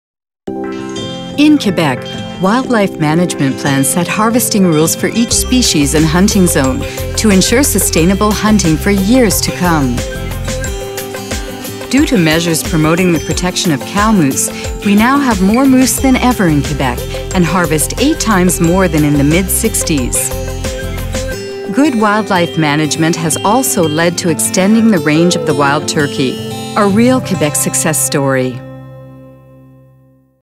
Hunting : wildlife management
This video enlights how a good wildlife management leads to a healthy and sustainable hunting for years to come.